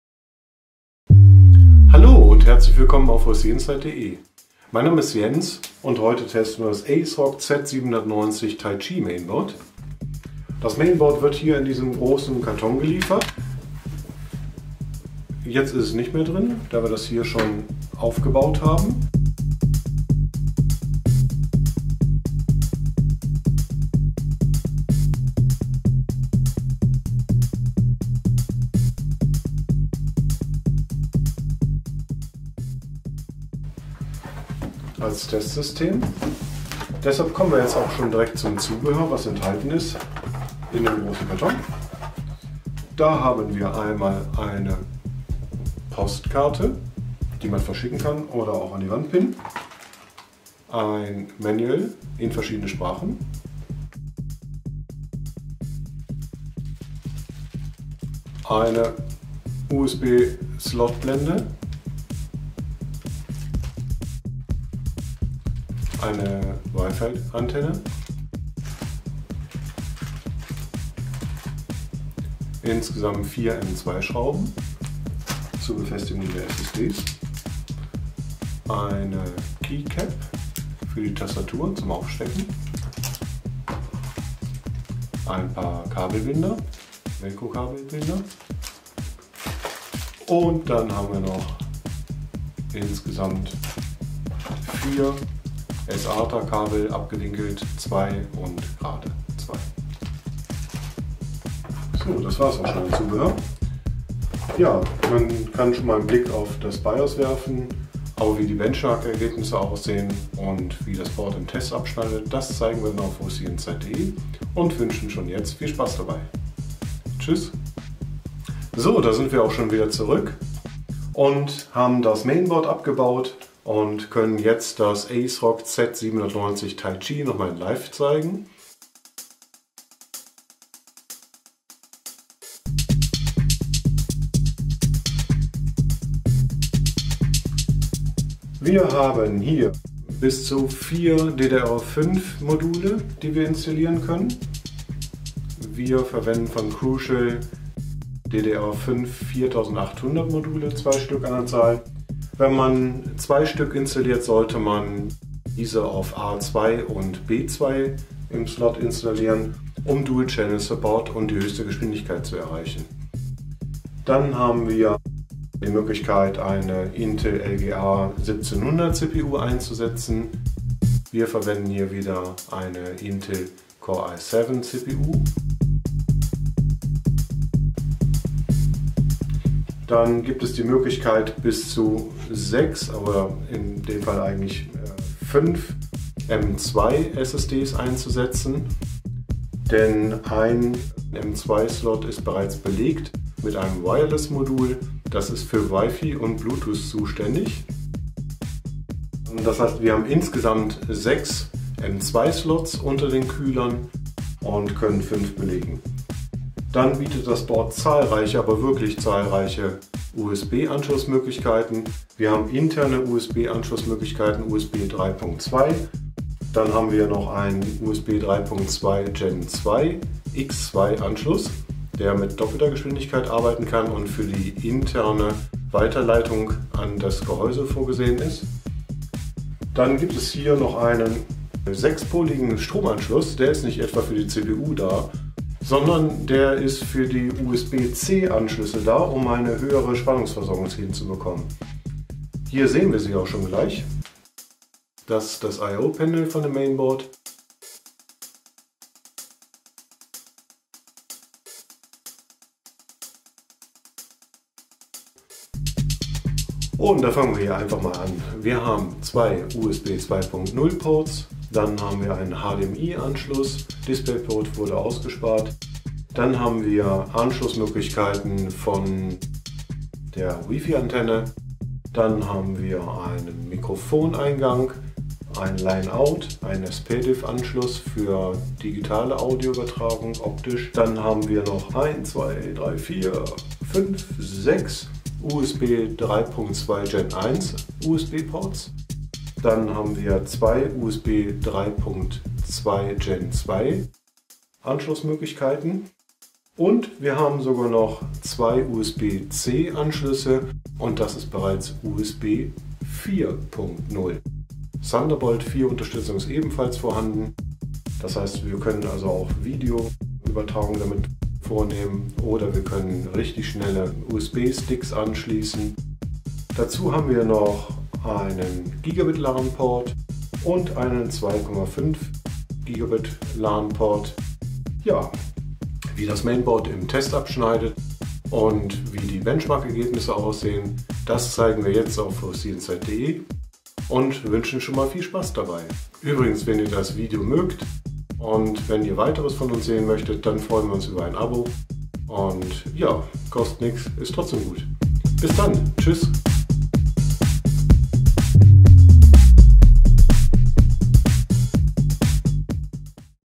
ASRock Z790 Taichi LGA1700 Mainboard Unboxing Video
Timestamps:
00:00 ASRock Z790 Taichi auspacken
00:35 ASRock Z790 Taichi Zubehör enthalten
01:42 ASRock Z790 Taichi UEFI
02:18 ASRock Z790 Taichi DDR5 Steckplätze
02:44 ASRock Z790 Taichi LGA 1700 Sockel
02:58 ASRock Z790 Taichi M.2 Steckplätze
03:31 ASRock Z790 Taichi USB intern
03:46 ASRock Z790 Taichi USB Stromanschluss
04:00 ASRock Z790 Taichi I/O Panel
04:57 ASRock Z790 Taichi USB 4.0 Thunderbolt 4 Ports
05:15 ASRock Z790 Taichi LAN Ports
05:32 ASRock Z790 Taichi Test auf OCinside.de
05:39 Danke fürs Abo & Like